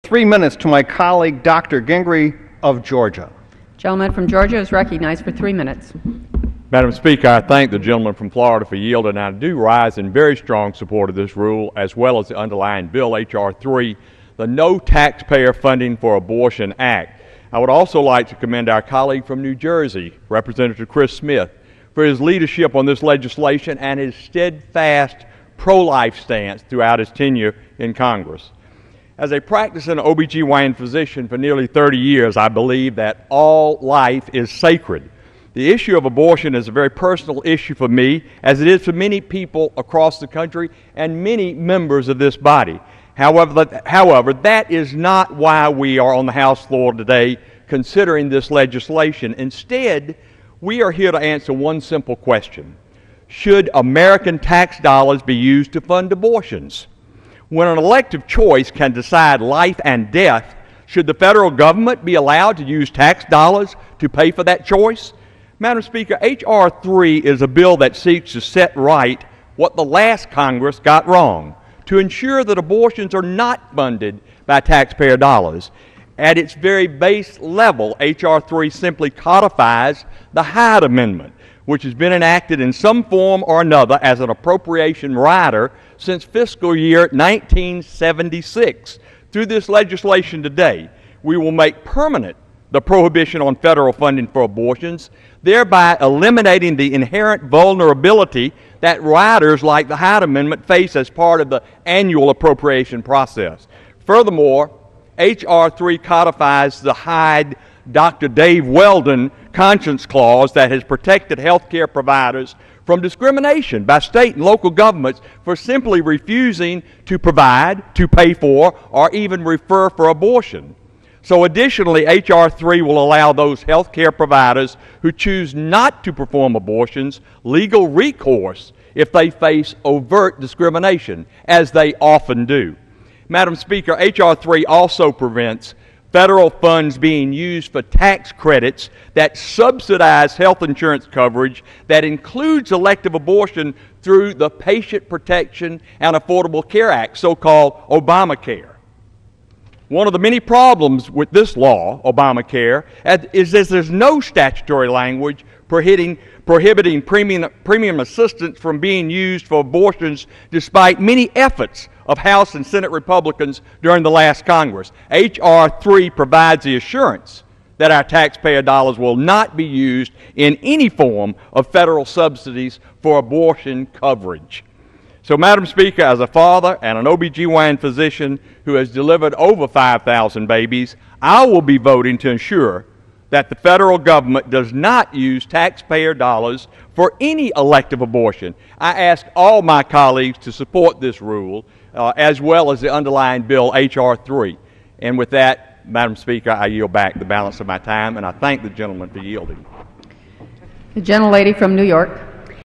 Floor speech in support of H.R. 3, the No Taxpayer Funding for Abortion Act
Congressman Gingrey speaks on House floor in support of the rule for H.R. 3, the No Taxpayer Funding for Abortion Act.